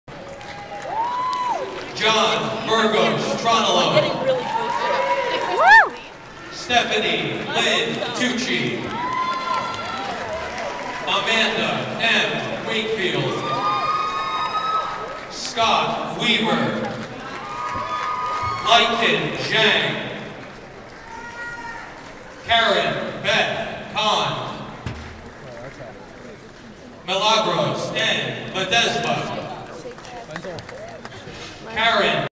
MU Graduation: "JBT"
After all those years, finally, my name is called - thanks to Christina G. for capturing this video! (May 20, 2010)